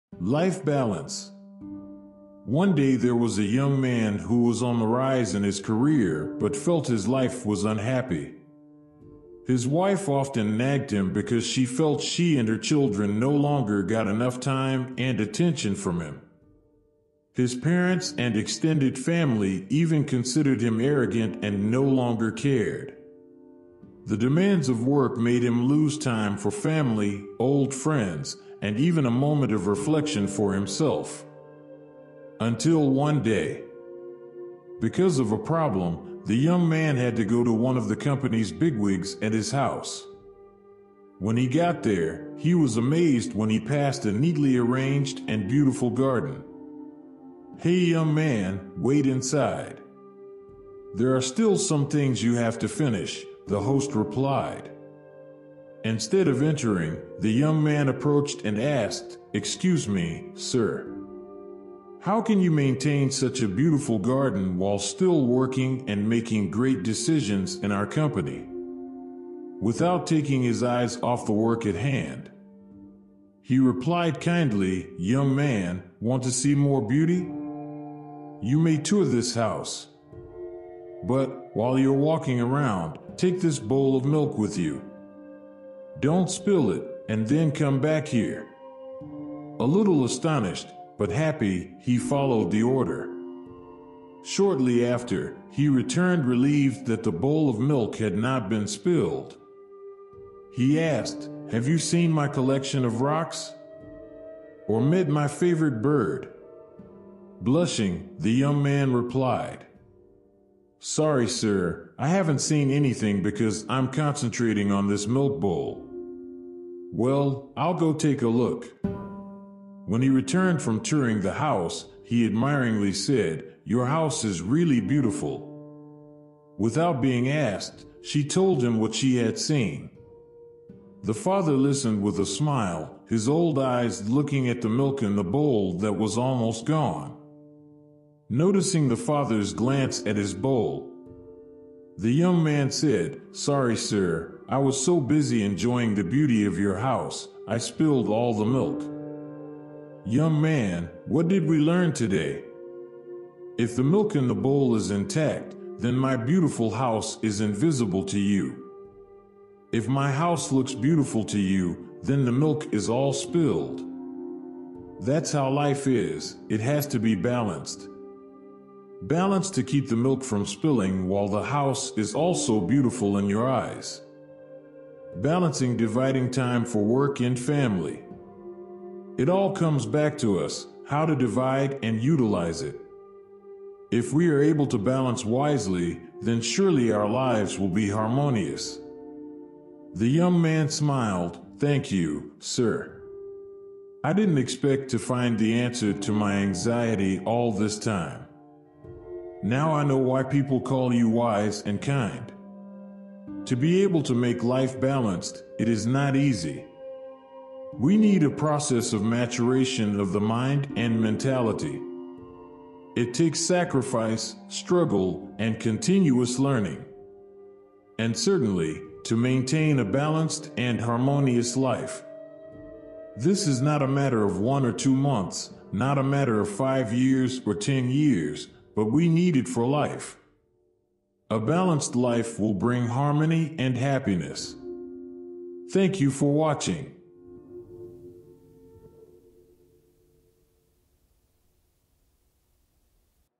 The Importance of Life Balance: A Story of Prioritizing What Truly Matters
The story is about a young man who is successful in his career but feels unhappy due to the demands of work that cause him to lose time for his family and personal life. One day, he visits the house of a company bigwig and is amazed by the beautifully arranged garden. The bigwig teaches him an important lesson about life balance by asking him to carry a bowl of milk while touring the house. Through this experience, the young man learns that life needs to be balanced to avoid spilling the milk and to appreciate the beauty around us. The story highlights the importance of balancing work and personal life, prioritizing what truly matters, and making ongoing efforts to achieve balance for a happier and more fulfilling life.